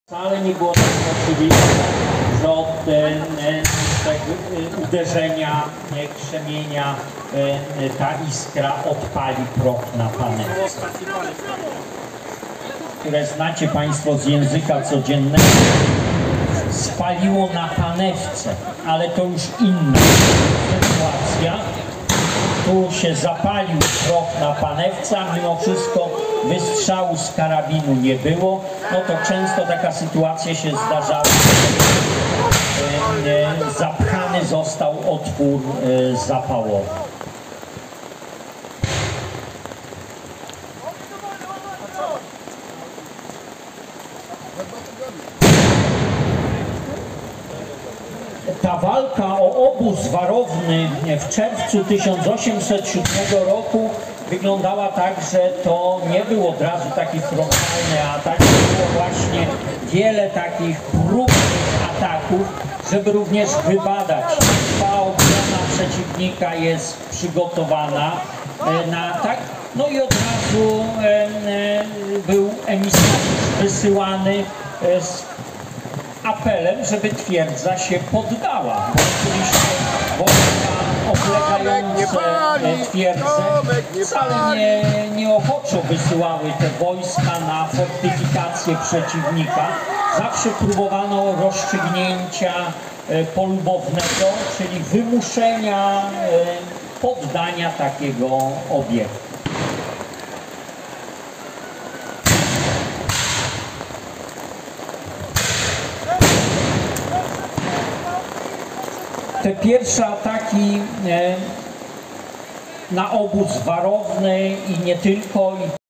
Dni Twierdzy Kłodzko 2023
Rekonstrukcja bitwy o Twierdzę Kłodzko z czasów napoleońskich.